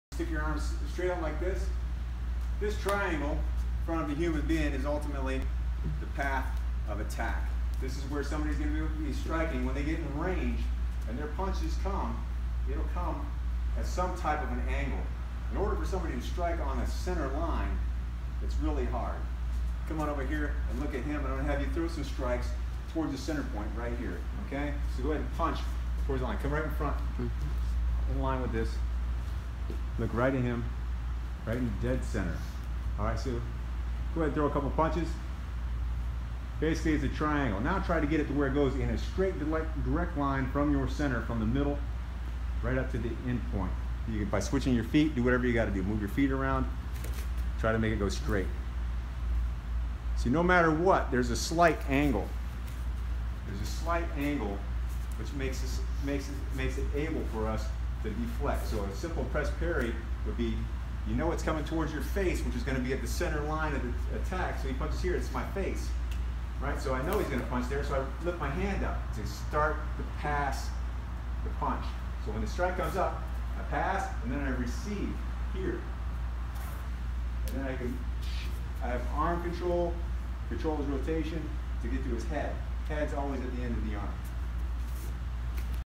Paradigm X Angles of Attack
Direct angles of attack you may expect to parallel, deflect, evade, parry, or redirect as you move off of the line of attack. The key point “triangle”